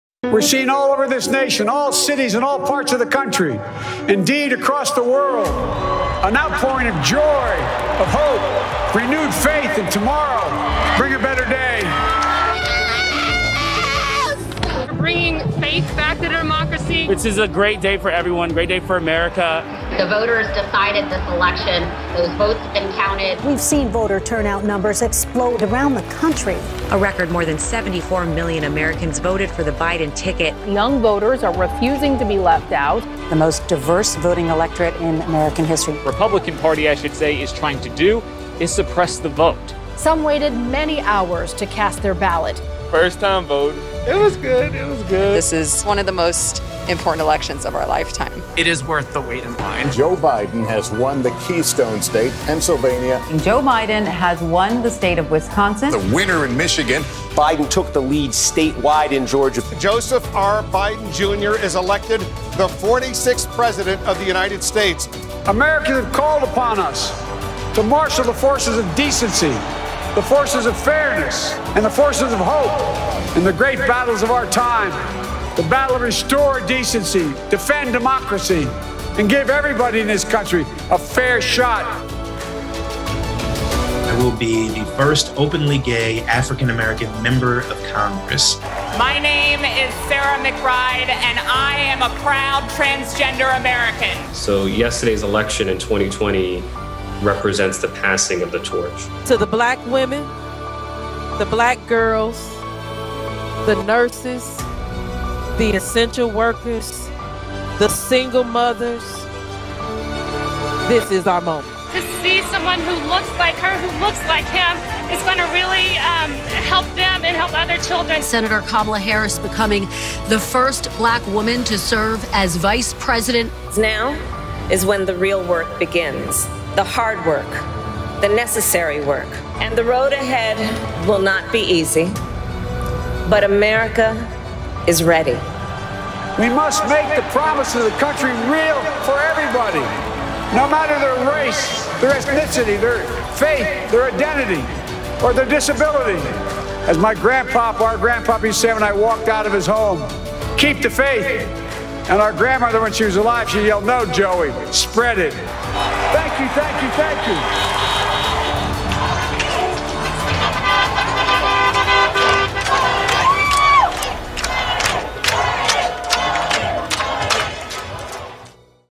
Voters Send Biden-Harris to the White House
An outpouring of joy has swept across the nation as voters send Joe Biden & Kamala Harris to the White House.